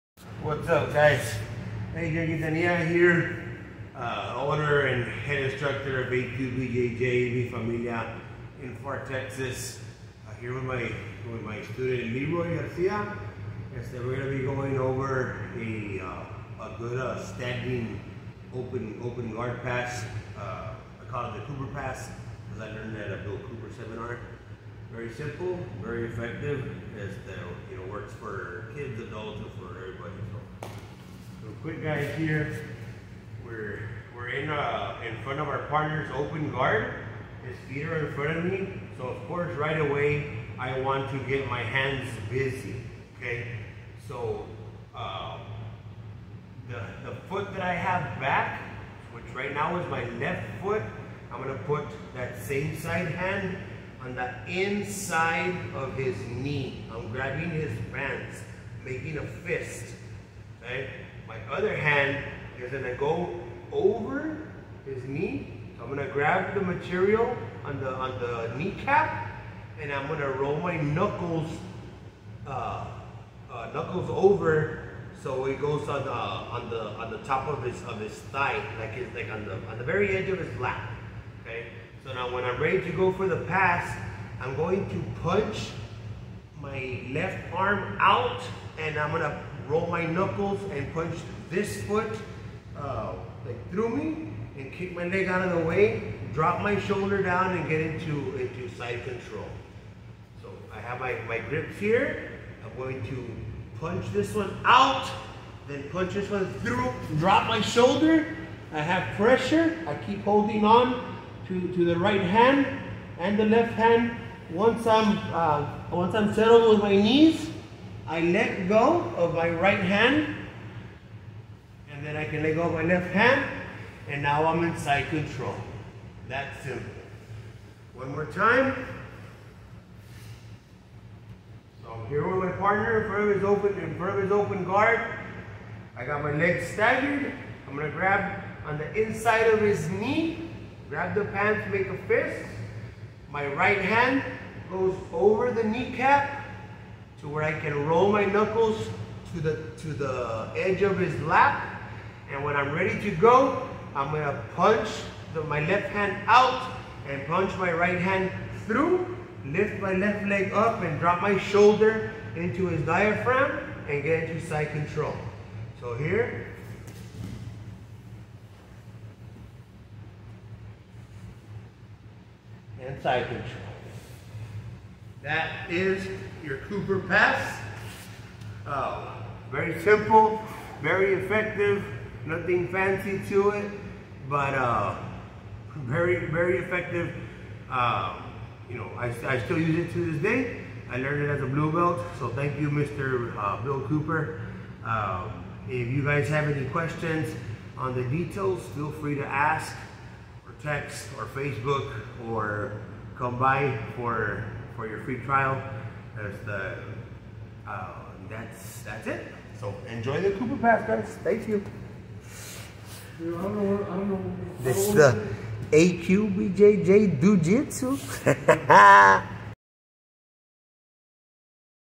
BillCooperPass OpenGuard aqbjj minhafamilia
The Cooper pass is on open guard pass. You want to grab their pants right on the inside part of the knee with your left hand and have that same side foot back. meaning you're right-footed to the front. with your right hand you grab on the kneecap and roll your fist so your knuckles are running the edge of their lap. And then when you are ready to pass you punch your left hand out so the leg doesn't come near you to try to recompose guard and with your right hand you use your knuckles to punch the knee when you roll your hand grabbing the pants. simultaneously you drop your shoulder as you pass through the outside. quick trip I forgot to mention have your right foot to the outside of their hips when they are on their backs or on their butt a little bit. your left leg comes up to help drive more weight through your shoulder on to their stomach and then you pass all the way to side control once you have both knees were they belong you can go ahead and let go of the right hand and circle awards towards the head and if their arm is in the way you can get that kind of head and arm side of control position or just move their arm and hug the head for traditional side of control and you can let go of the other side and underhook their forearm and Gable grip your hands so you can establish a tight side of control.